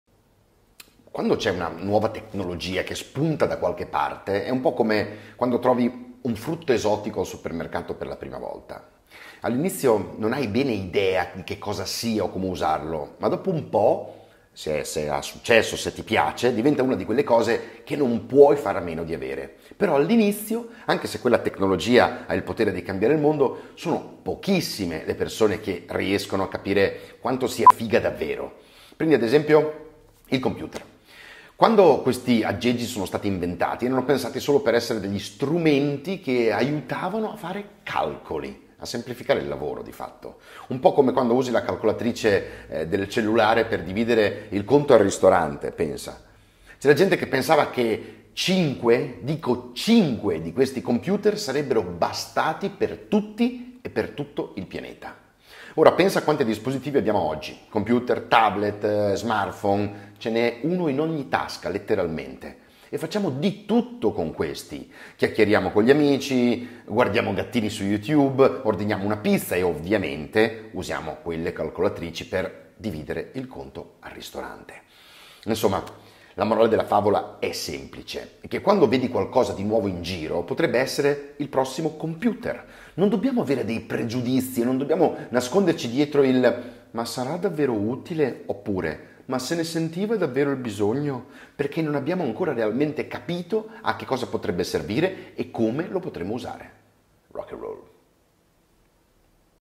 Una nuova tecnologia è come un frutto esotico al supermercato
Quando c'è una nuova tecnologia che spunta da qualche parte, è un po’ come quando trovi un frutto esotico al supermercato per la prima volta: non sai bene cosa farci